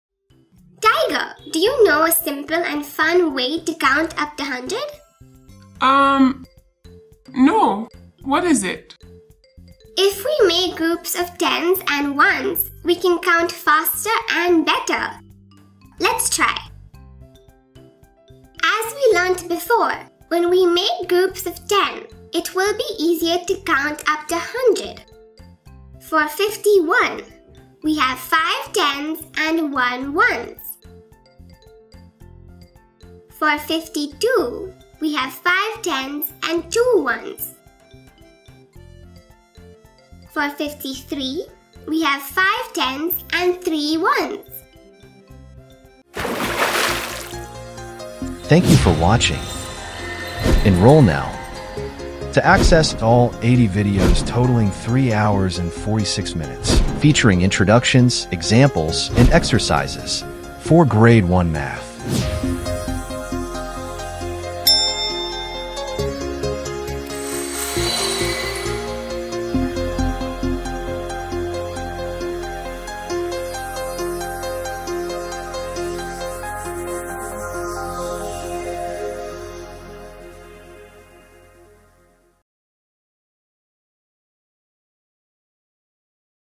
G1 - Module 16 - Exercise 1a - Make 10s and Count | Appu Series | Grade 1 Math Learning
What’s the secret to making 10s and counting? Join Appu and Tiger on this exciting adventure to uncover it! You’ll explore fun ways to create groups of 10 while enhancing your counting skills. Learn well and have fun along the way!